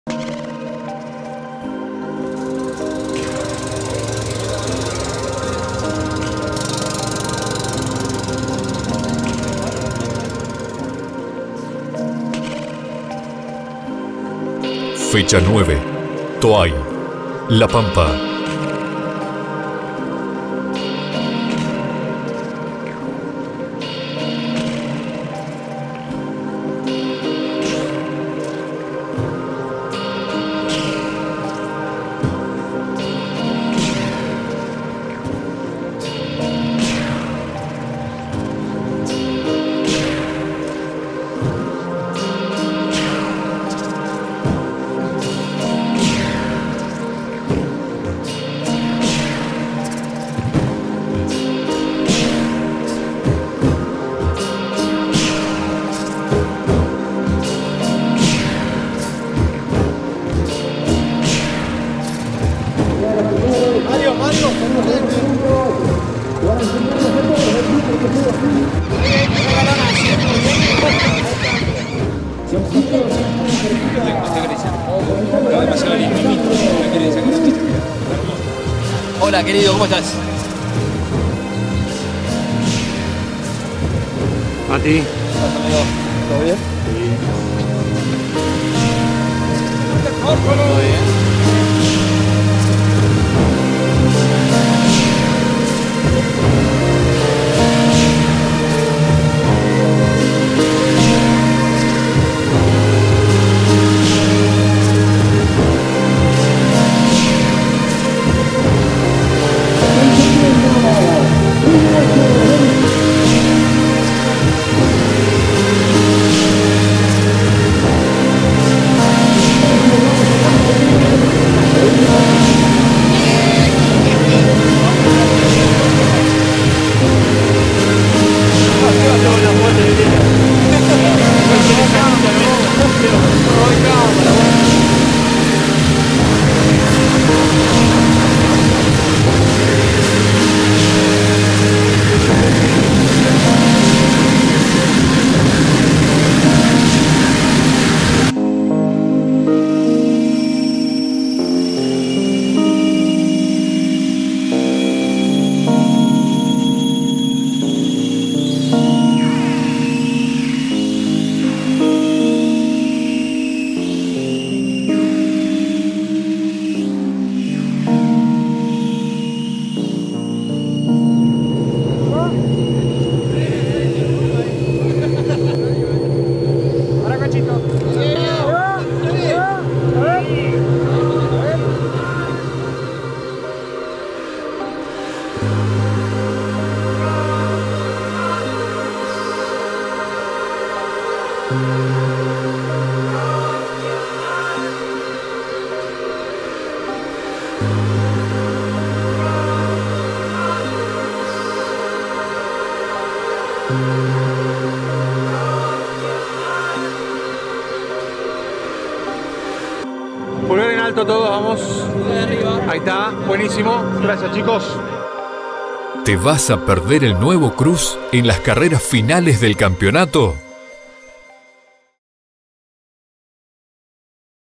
Cómo se hizo la foto del NuevoCruze del EQUIPO CHEVROLET YPF de STC2000
Mirá, así se produjo la foto del EQUIPO CHEVROLET YPF con los pilotos y los dos NuevoCruze que debutaron en el autódromo pampeano de Toay. Coo siempre, te mostramos el detrás de escena, lo que nadie te muestra.